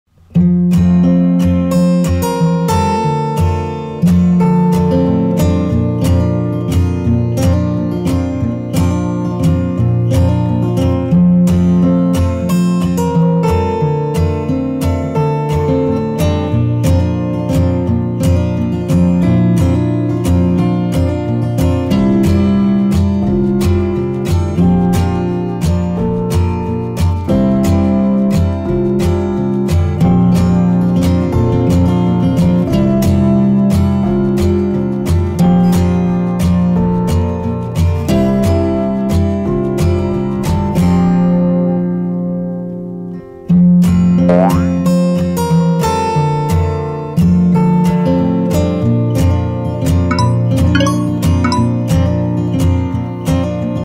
Change objects color - Adobe Photoshop tutorial
Photoshop Tutorial - Change objects' color.
learn how to change objects' color in Adobe Photoshop by using Hue and Saturation to object color in Adobe Photoshop.